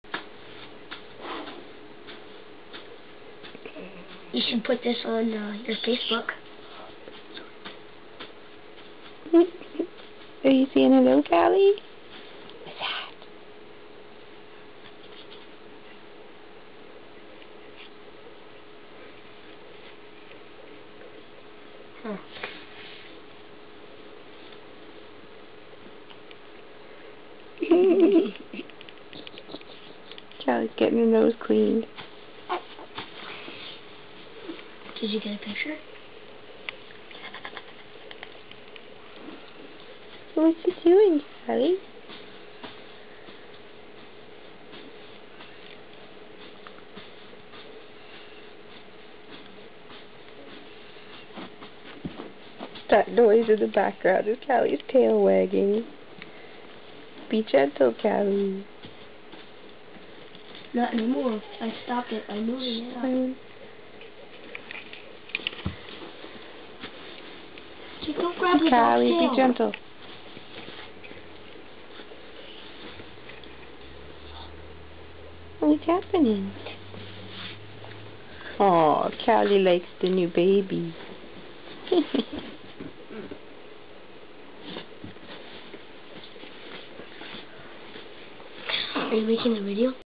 Cali the dog playing with Edgar the rat
The rat came to our house the other day, one of two for the purpose of being fed to Hazel the ball python, but lasted in the cage overnight so we've adopted it.  The dog is Cali, our Goldenbox (half retriever, half boxer).